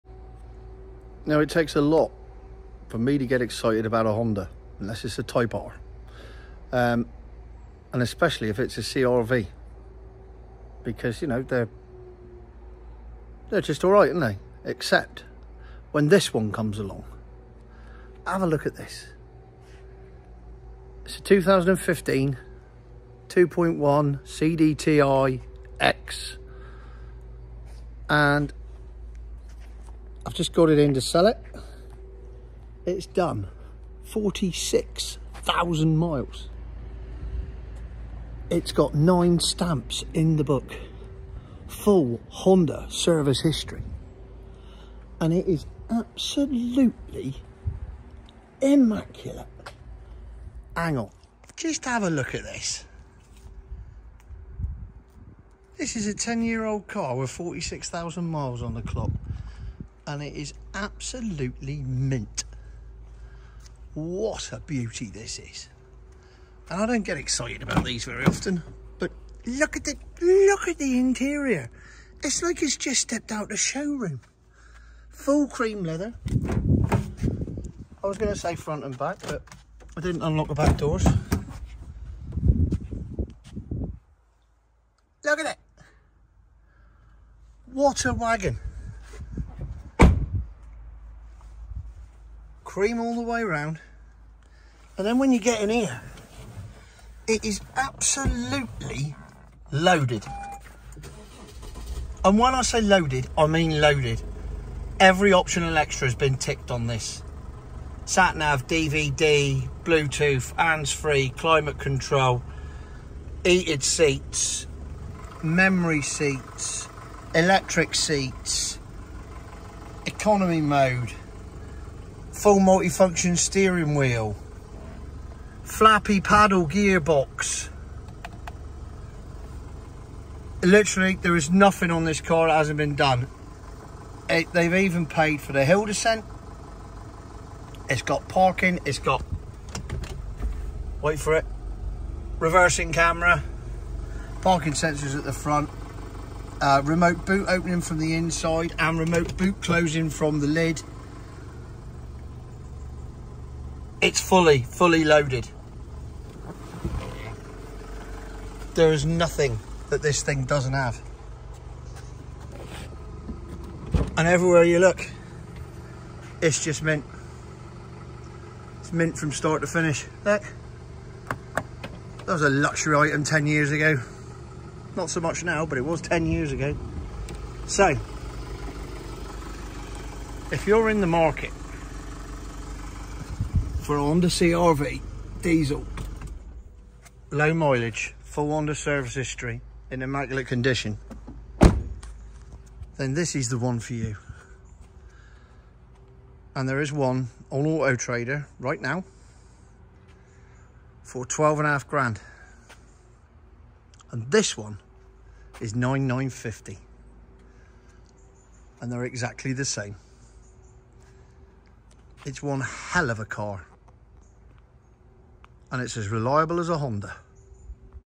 2015 HONDA CR-V I-DTEC EX 46K FSH AND SHES ABSOLUTELY IMMACULATE!!
Honda CRV Ex I-DTEC Auto

2015

46000 miles

MOT till May 2025

FULL MAIN DEALER SERVICE HISTORY (last service at 42K) with 9 stamps in the book

Immaculate condition inside and out

2 keys

Honda Owners Pack

This Honda CRV comes fully loaded and when I say fully loaded check out this list!!!

Full Cream Leather Interior in fantastic condition
Heated Seats
Memory Seats with 2 presets customisable to each key
Electric Windows
Electric Mirrors
Cruise Control
Climate Control
Sat Nav
DAB Radio
DVD player
Bluetooth and Handsfree
Auto Headlights
Auto Wipers
Eco Mode
Reversing Camera
Front Parking Sensors
Panoramic Roof
Electric boot closing
Flappy paddle gearbox
Hill Descent

It even comes with the original Honda CRV mats front and rear. This car is an absolute credit to its previous owners. One of  the finest examples of this model on the road. What an absolute beauty.

£9950 Ono

No part ex or swaps on this one I’m afraid.